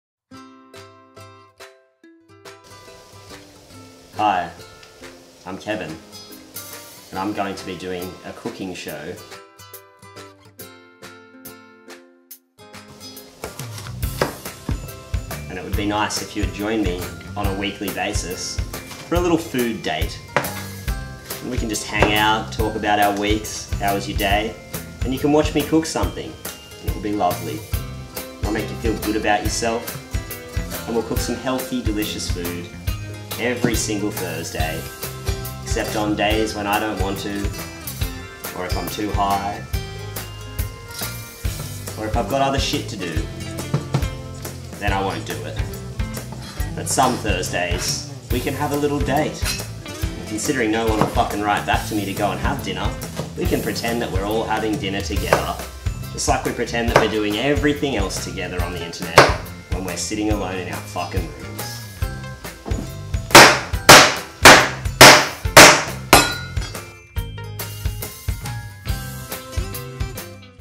Cooking with Kevin (OFFICIAL TEASER TRAILER)
Come with me on a magical journey through the foodscape to a land of pure and unadulterated gustatory and olfactory bliss on Cooking with Kevin. Episodes coming soon if I have it in me.